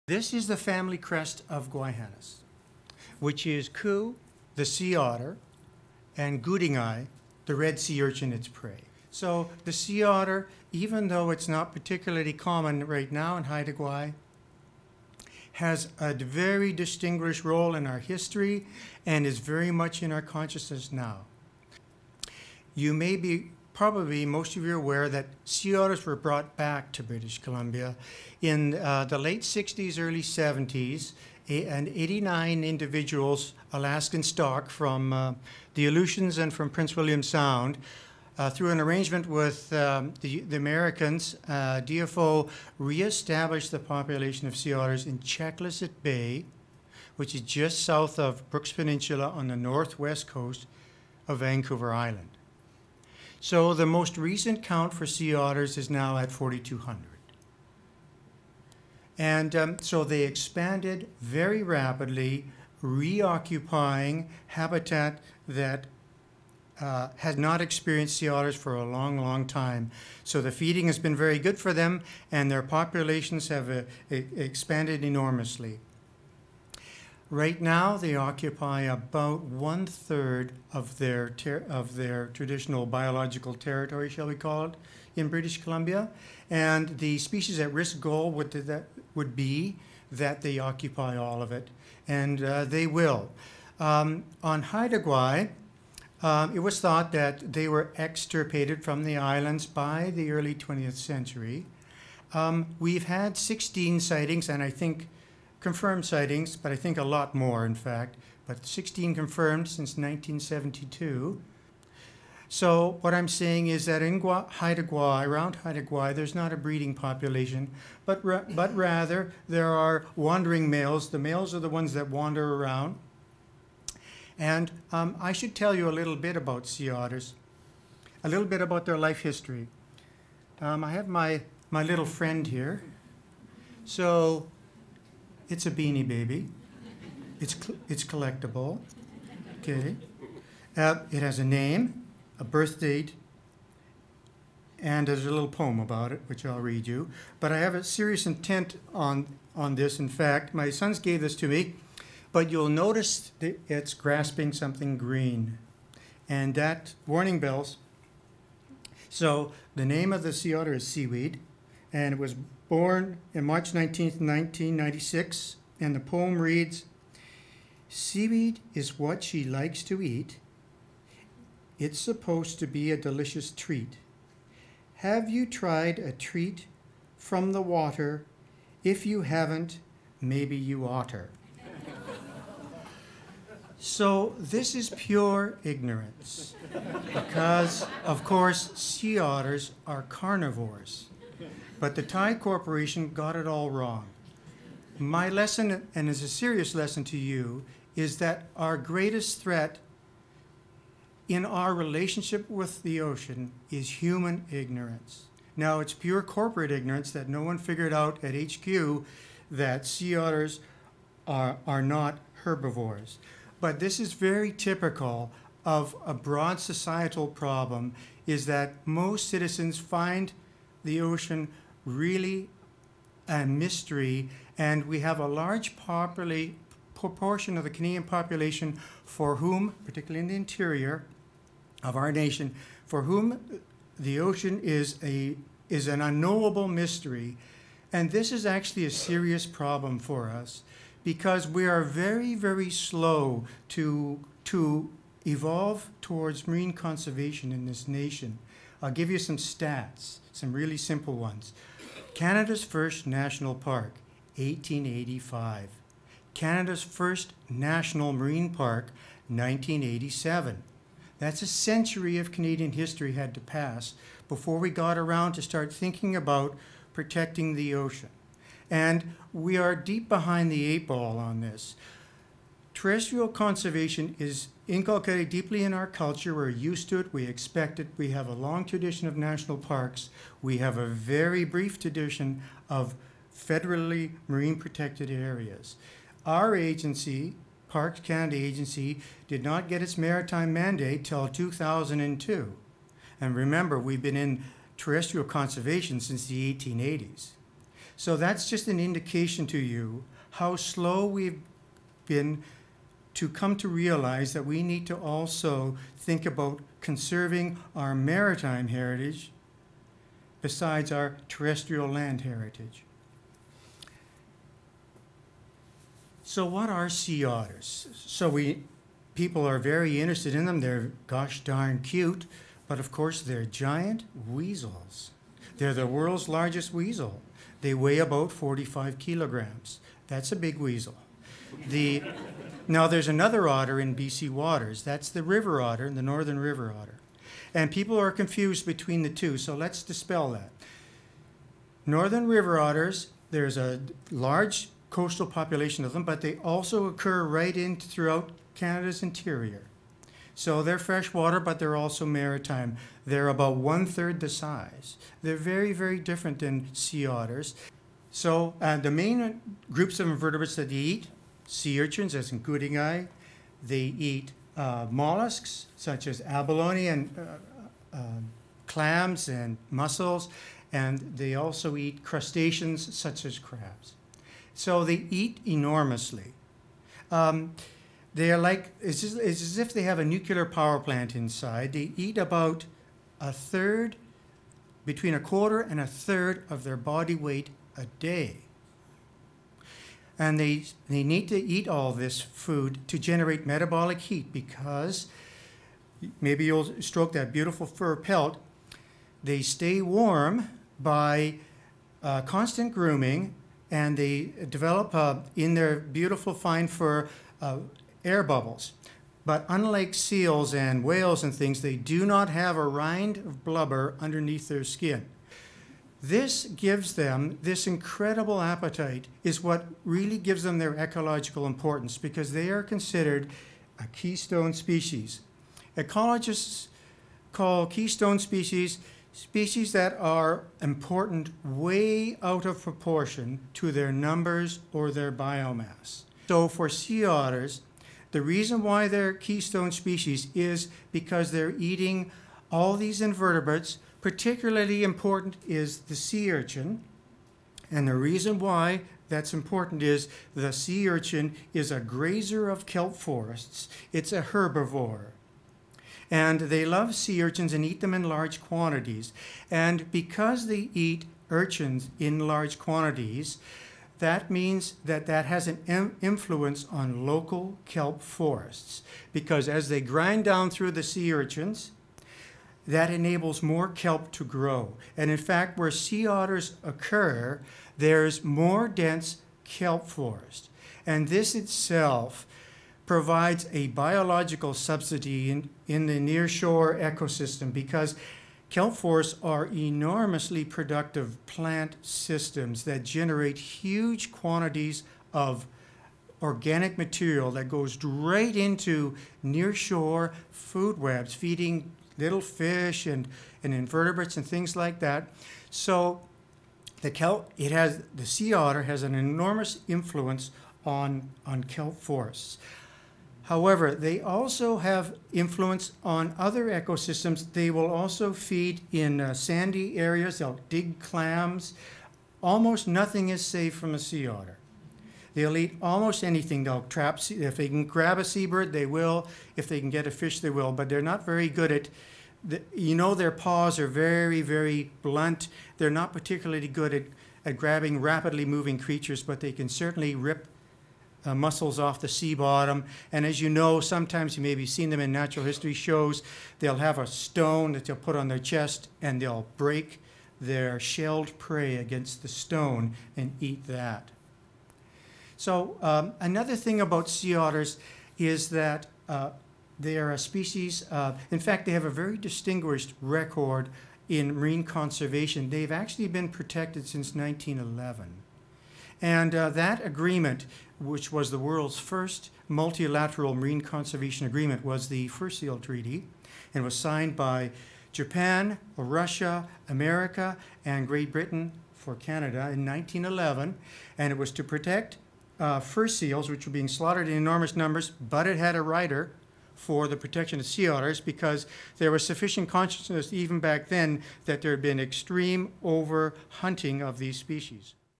Stories from Gwaii Haanas - Dr. Norm Sloan: Sea Otters of Haida Gwaii
Stories from Gwaii Haanas - Join Norm and Lyle for a fascinating exploration of the history and ecology of the sea otter on Haida Gwaii over the last two centuries. Learn about what the ecosystem would have been like at the advent of the fur trade, and the implications of fur trade harvest on the abundance of sea otters.